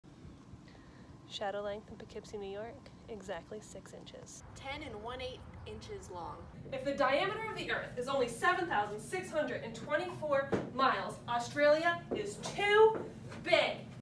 GISH2018 Team MOTHRA - Item 163
The Flat Earth society says Australia doesn’t exist. Prove them right (or wrong.) Perform an experiment to prove your hypothesis! Bonus points if you can measure the diameter of the Earth to within 10% of its actual value. No fudging numbers – just like in school, you have to show how you arrived at your answer! - Bobak Ferdowsi, System Engineer, NASA Jet Propulsion Laboratory